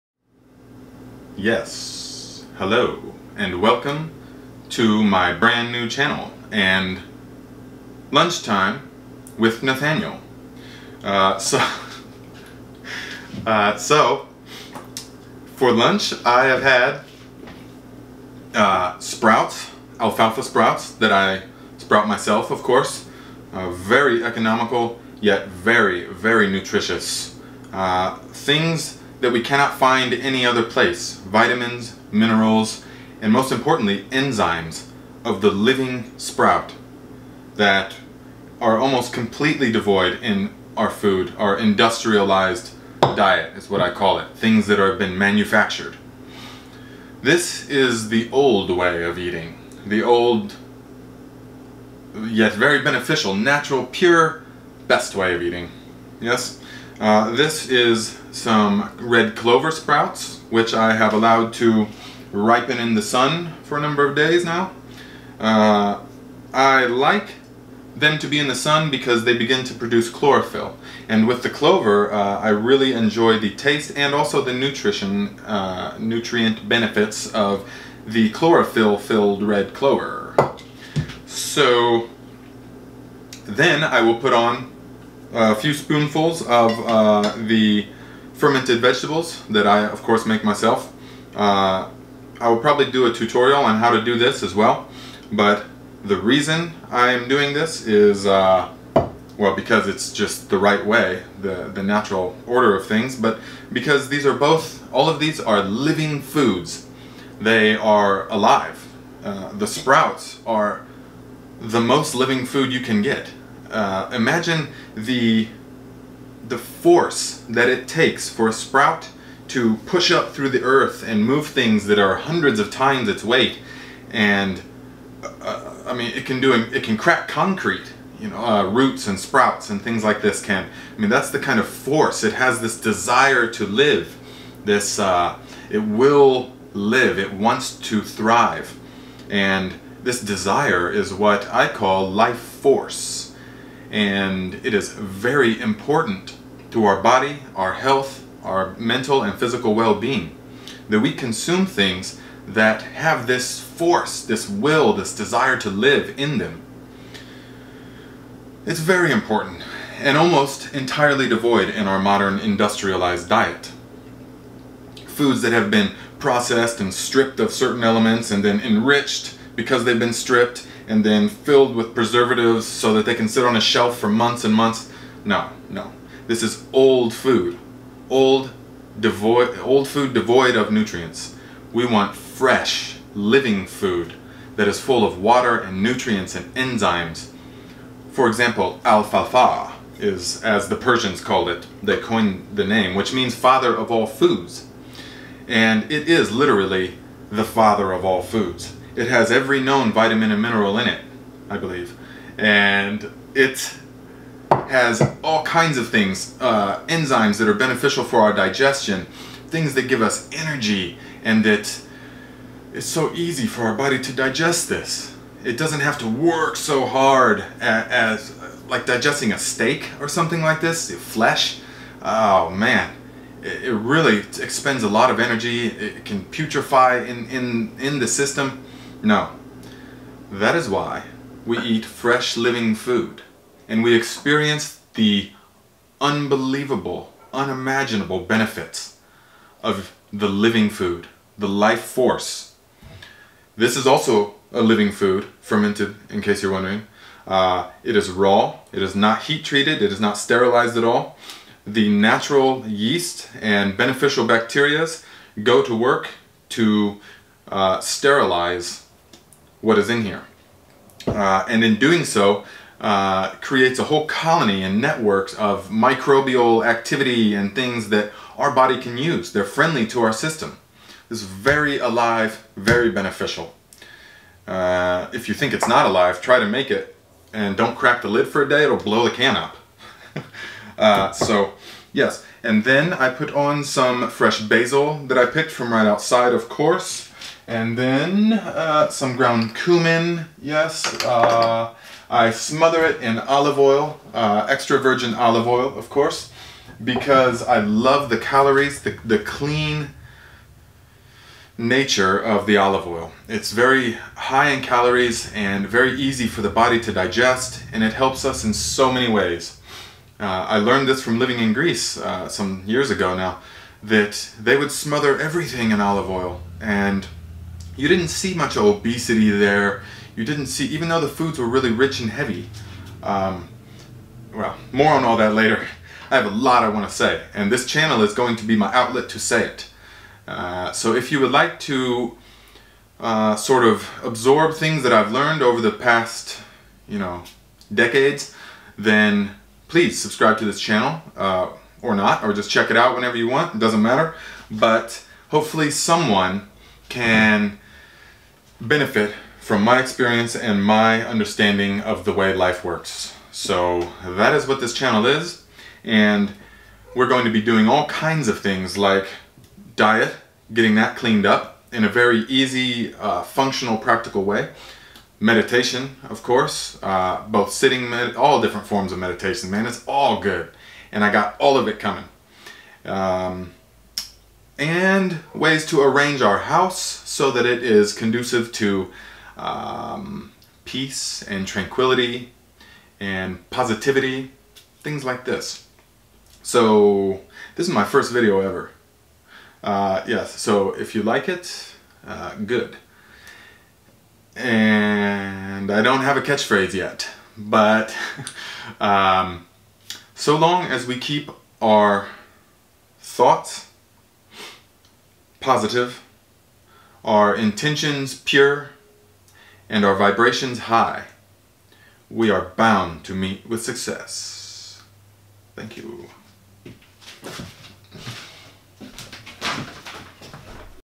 Eat Sprouts everyday for better health
Make Sprouts a foundational part of your diet and watch your health improve dramatically!.. This video explains how to make raw sprouts a daily part of your diet and a bit of the how and why I eat this way and some of the benefits that come with it... many more videos on the way.